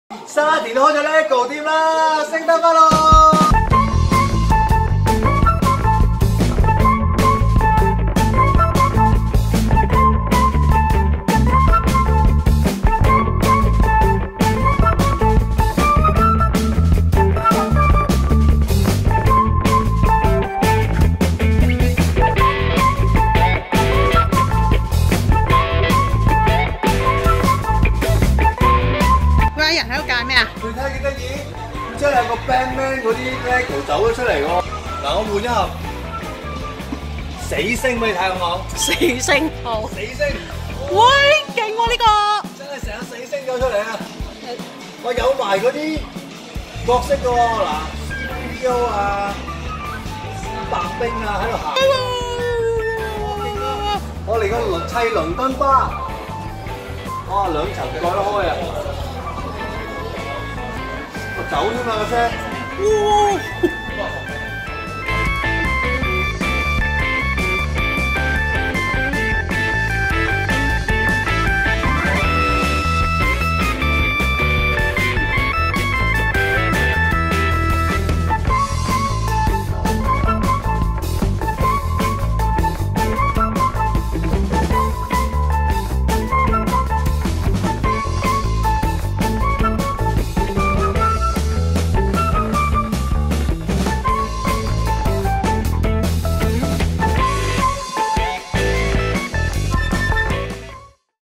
【免費玩Digital Box】LEGO Store 第三分店開張~無限送Lego聖誕卡
LEGO Store
地址：沙田新城市廣場一期 4 樓 416 舖
營業時間： 10am - 10pm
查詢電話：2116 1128